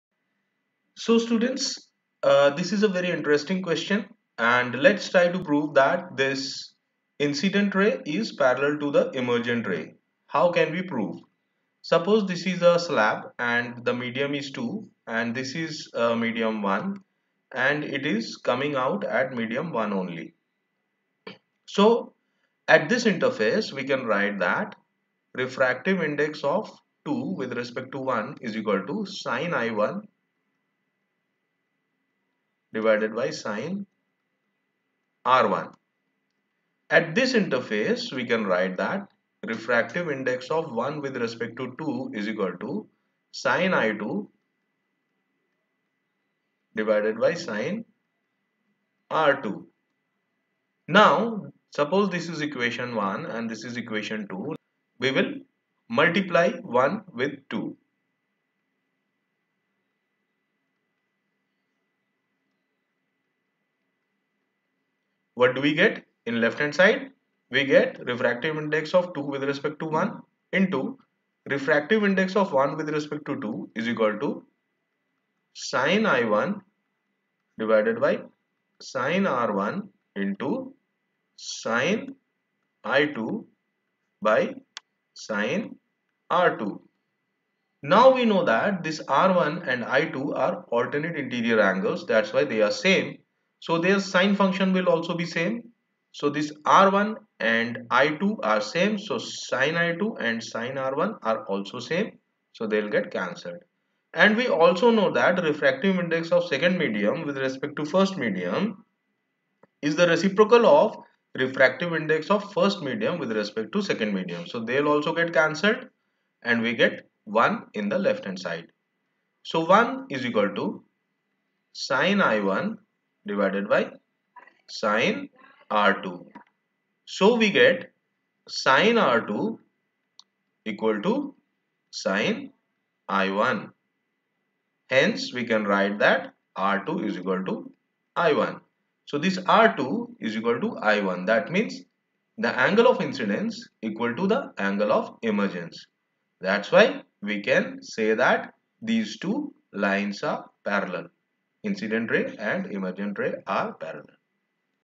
Derivation of incident ray is parallel to emergent ray in refraction
Explanation and derivation of why incident ray is parallel to emergent ray in glass slab